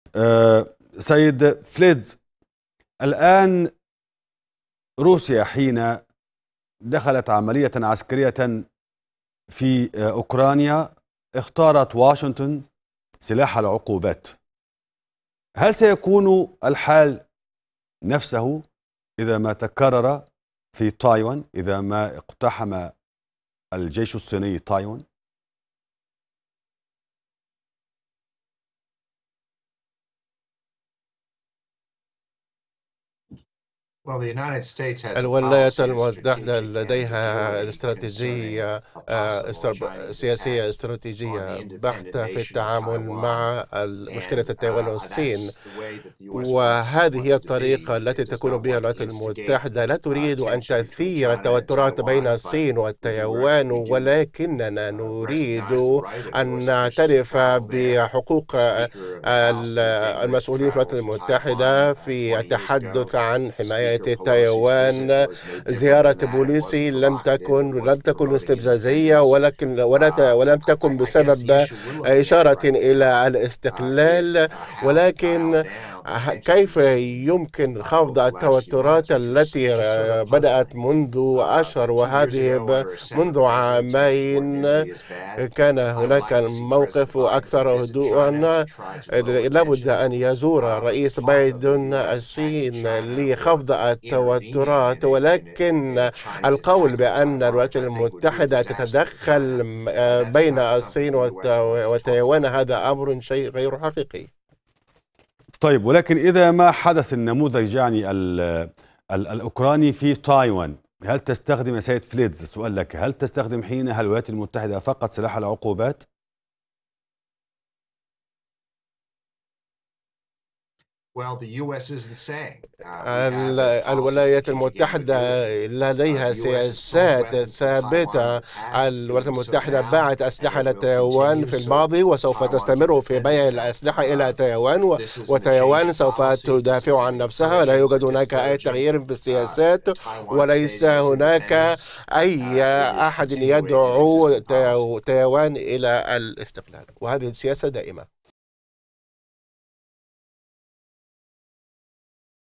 في حال قيام الصين بعملية عسكرية في تايوان.. هل تتحرك أمريكا؟
العملية_العسكرية_الروسية لم تؤثر فقط على أوكرانيا أو أوروبا ، بل امتد تاثيرها لخمسمئة ميل و وصل إلى جزيرة تايوان..
فالرئيس الصيني يتحدث عن تايوان كما يتحدث  الرئيس الروسي  عن أوكرانيا .
غير أن الصين دائما ما تقول إن تايوان ليست أوكرانيا.. لأن الأولى تعد تاريخيا جزءا لا يتجزأ من جمهورية الصين الشعبية..
كما أن  الصين تفترض أن الدعم العالمي لتايوان سيكون أقل صخبا مما كان عليه لأوكرانيا، لأن دولا قليلة  تقيم علاقات دبلوماسية مع تايبيه.
تفاصيل أكثر عن الموضوع مع ضيف قناة الغد من  واشنطن، فريد فليتز، مسئول سابق في مجلس الأمن القومي الأمريكي.